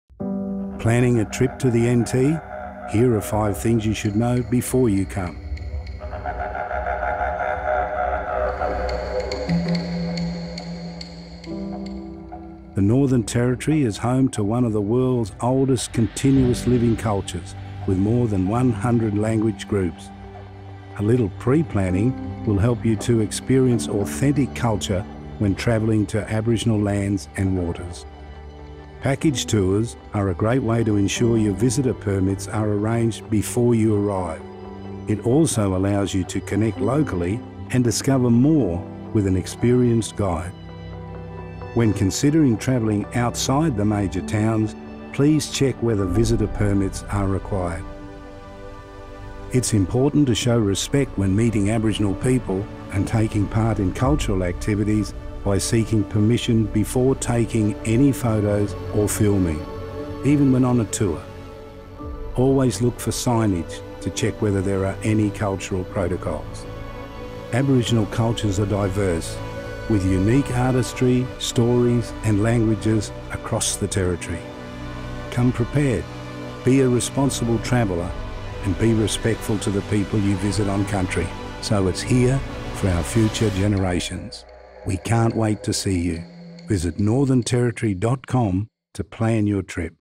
How to travel responsibly on Country in the NT
The Northern Territory is home to one of the world’s oldest continuous living cultures with more than 100 language groups throughout the regions. Here are five tips to help you pre plan your holiday and travel responsibly when visiting Aboriginal lands and waters in the Top End and Red Centre.

Narrated by Charlie King